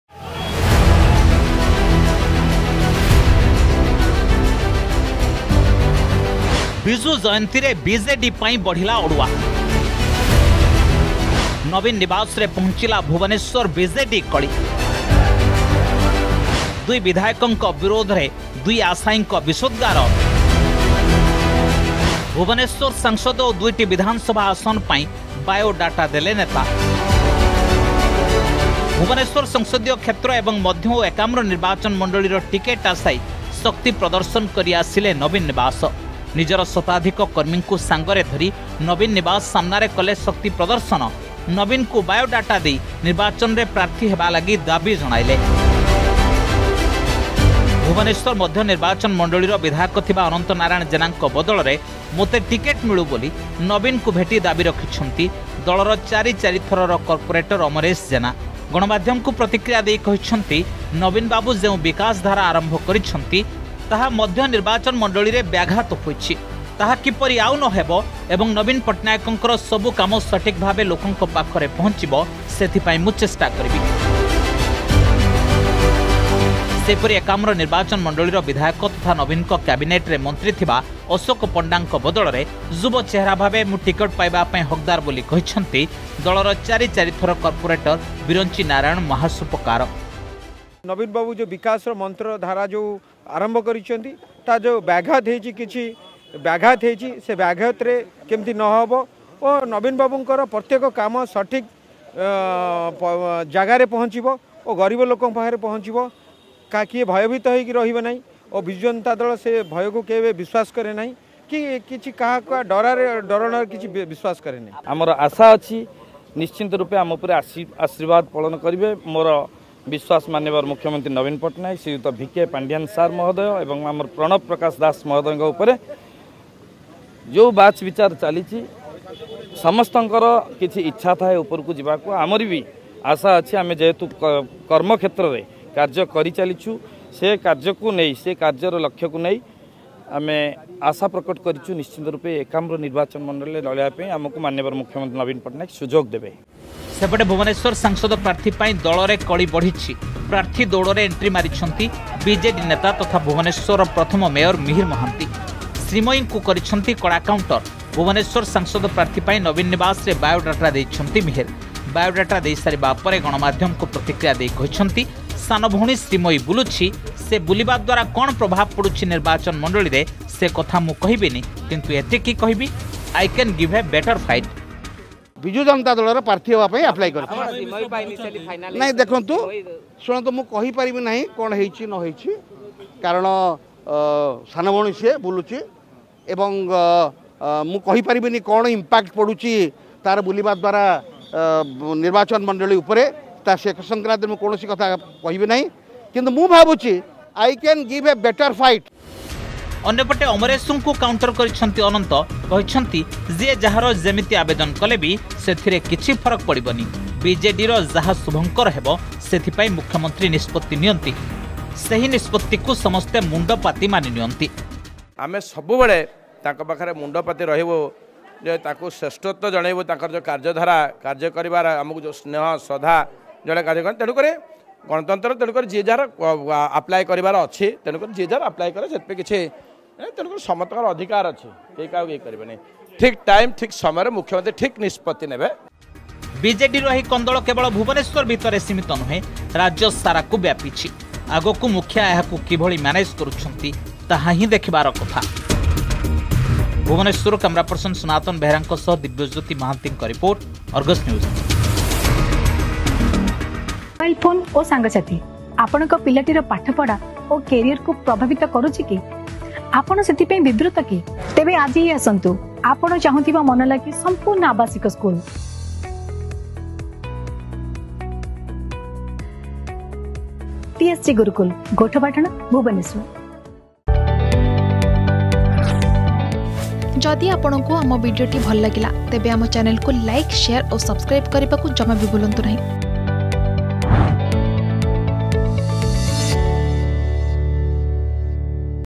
ବିଜେଡିରେ ଅସମ୍ଭାଳ ଗୋଷ୍ଠୀ କନ୍ଦଳ || Bhubaneswar BJD Controversy
ବିଜୁ ଜୟନ୍ତୀରେ ରାଜଧାନୀରେ ଶାସକ ଦଳ ପାଇଁ ବଢ଼ିଲା ଅଡୁଆ । ନବୀନ ନିବାସରେ ପହଞ୍ଚିଲା ଭୁବନେଶ୍ବର ବିଜେଡି କଳି । ଦୁଇ ବିଧାୟକଙ୍କ ବିରୋଧରେ ଦୁଇ ଆଶାୟୀଙ୍କ ବିଷୋଦଗାର ।  ଲୋକସଭା ଓ ଦୁଇଟି ବିଧାନସଭା ଆସନ ପାଇଁ ବାୟୋ ଡାଟା ଦେଲେ ନେତା ।